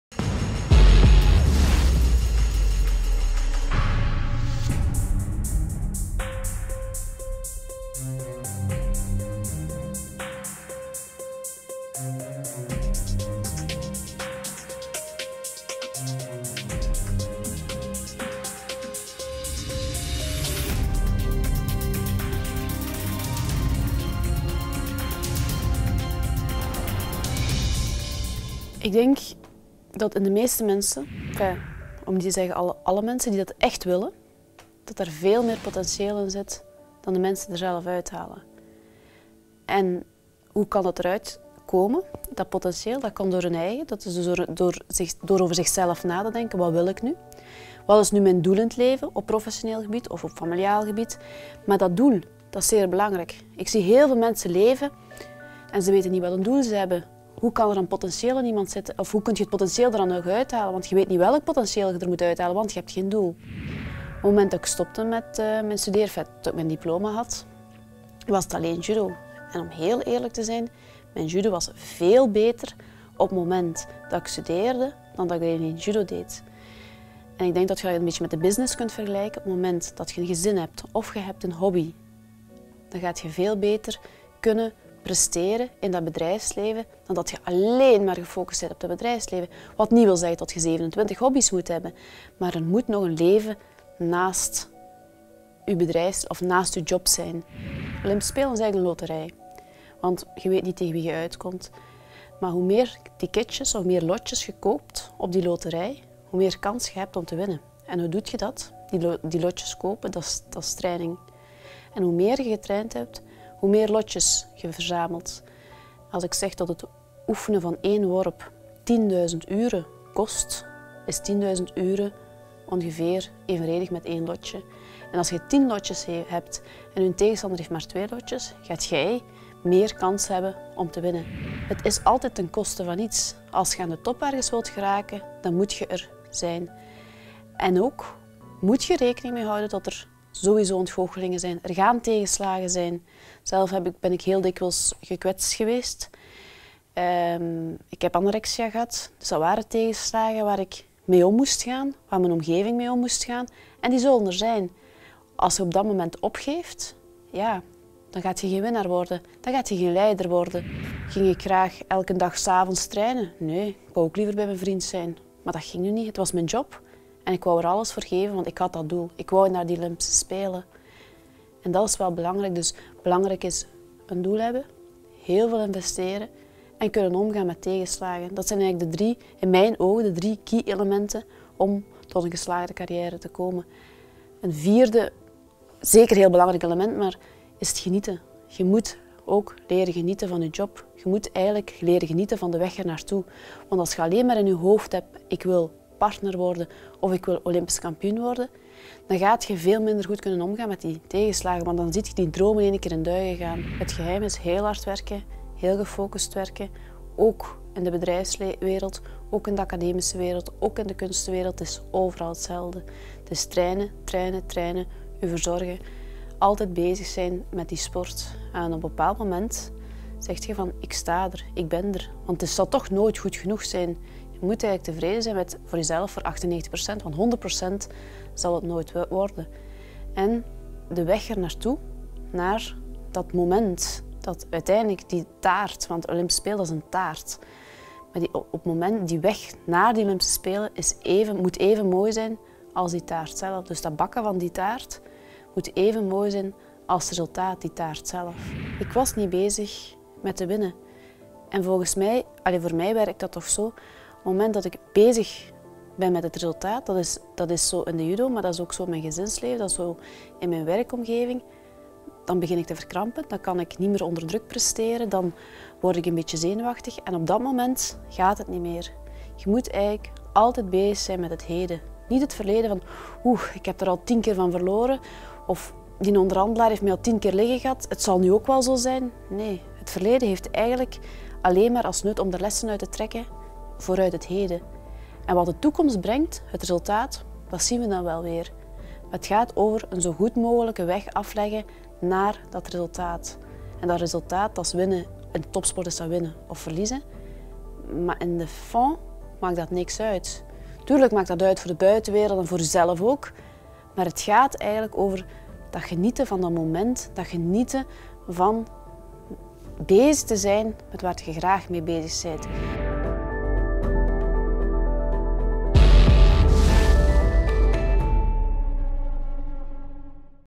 Ann Simons - High Performer
Ann Simons, former professional judoka and business coach, speaks of what it takes to reach the top.

Subtitles available in English, French and Spanish.